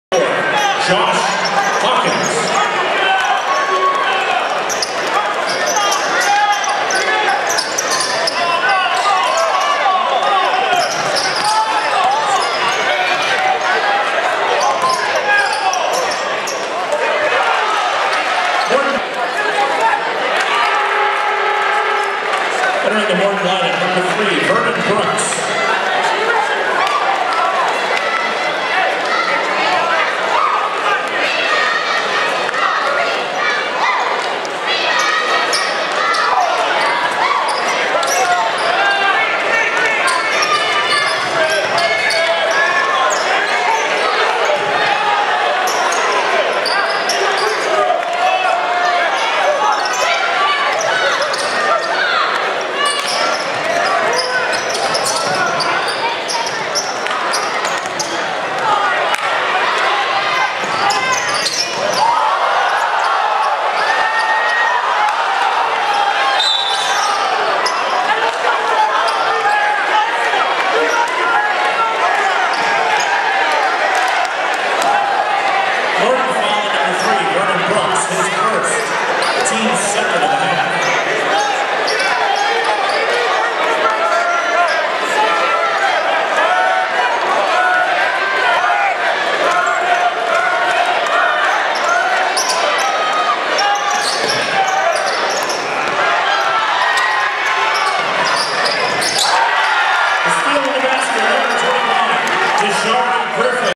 Morton Vs Gavit in the Rumble at the Civic Center
First Vernon Brooks splits two defenders. Second Vernon break the defender ankles